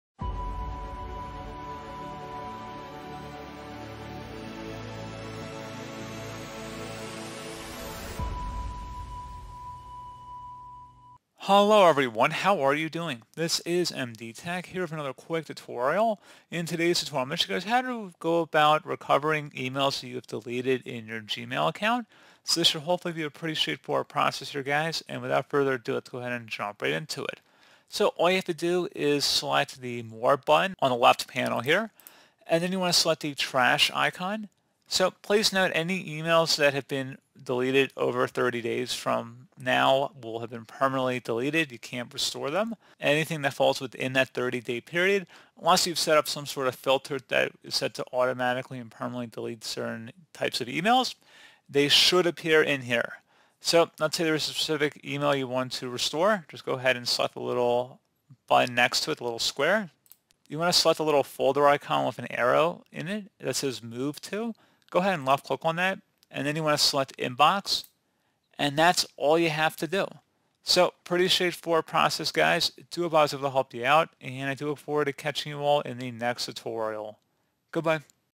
How to Recover Your Deleted Emails in Gmail [Guide]
We’ve all done it: inadvertently hit Delete on an important email and condemned it to the scrap heap. Thankfully, Google knows that this happens and has made it possible for you to recover deleted messages in just a few simple steps.

how to recover deleted archived emails in Gmail